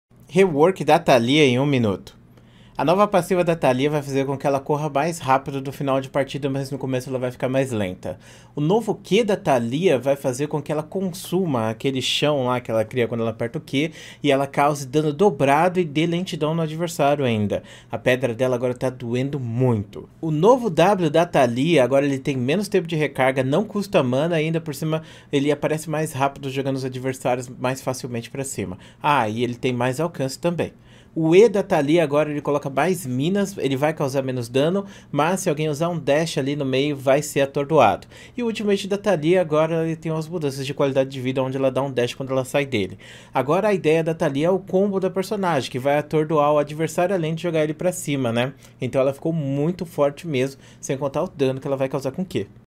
REWORK DA TALIYAH EM 1 MINUTO
shorts ai do rework da nossa querida conjuradora de pedras